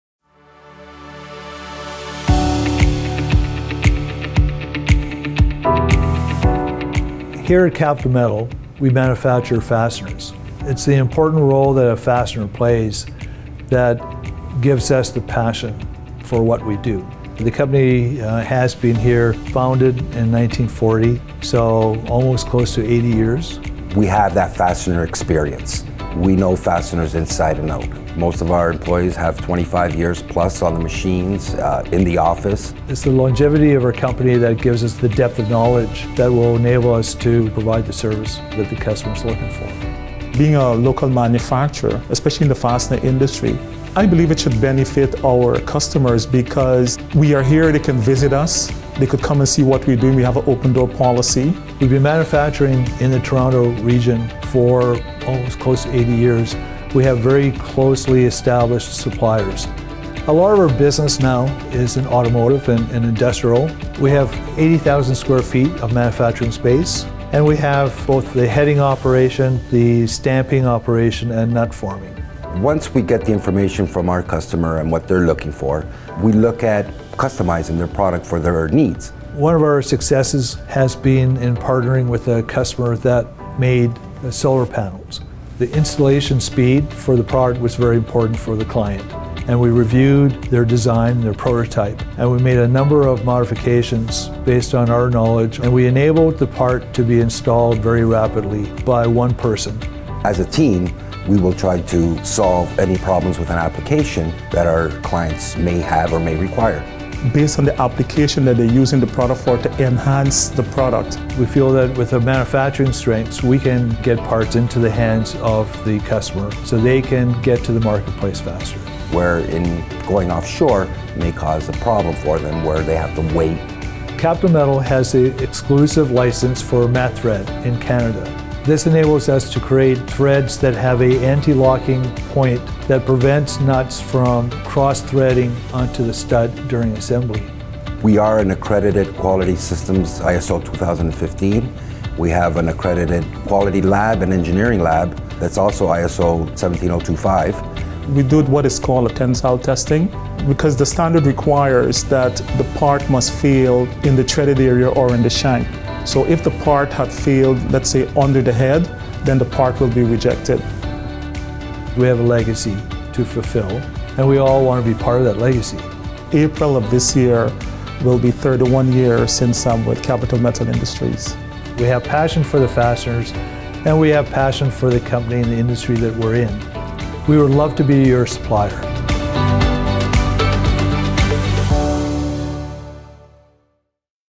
Capital Metal Industries (CMI) Manufacturing Capabilities
PRODUCTION CAPABILITIES:

Extruded Cold formed Sleeves
Carriage Bolts
Leg levelers
SEMS
Weld Bolts
Weld Studs
Pins
Dog Point Bolts
Wheel Bolts
Stamped Weld Nuts
Washers
Weld Nuts
Weld Spacers
Round Weld Nuts
Round Clinch Nuts

QUALITY CONTROL & ENGINEERING TEST LABORATORIES:

CMI’s test facilities are capable of testing all aspects of mechanical performance of fasteners. Tests are performed for core hardness, tensile strength, shear, salt spray, pull-out force and drive just to name a few. These tests are done not only in support of our automotive manufacturing facility at Capital Metal, but also in support of the fastener new product engineering and product testing for The Hillman Group.
New product engineering work includes testing existing Hillman fasteners with our competitor products. It is critical that our comparisons are done with the proper industry accepted methods and are consistently accurate. These tests are the springboard for the development of future designs that will address competitive issues. CMI’s accredited labs provide the confidence that these tests are concise, and that new designs can be equally validated for communication to our customers.